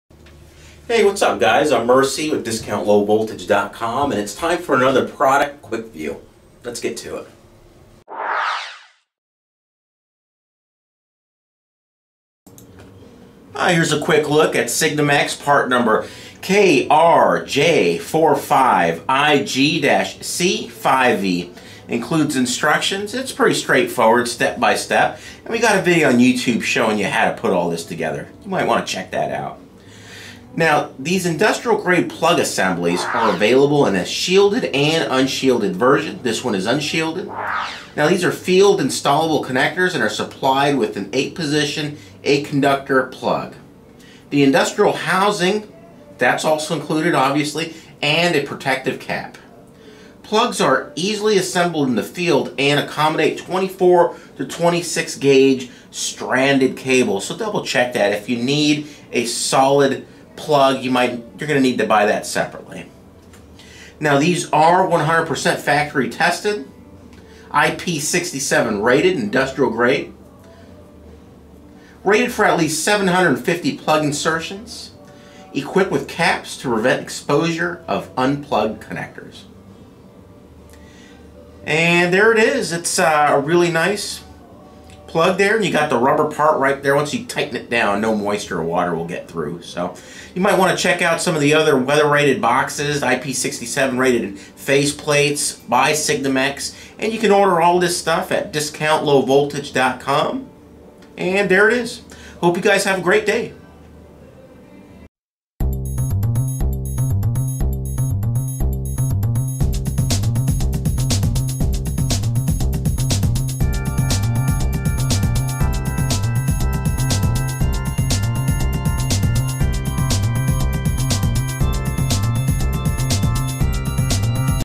Signamax KRJ45IG-C5E Field Installable Industrial Grade RJ45 Cat5e Modular Plug
Signamax KRJ45IG-C5E Cat5e RJ45 Industrial Grade Plug Assemblies are available in an unshielded (UTP) and shielded (FTP) versions. These field-installable connectors are supplied with an 8P/8C plug, industrial housing and a protective cap. Plugs are easily assembled in the field and accommodate 24- to 26-AWG stranded cable.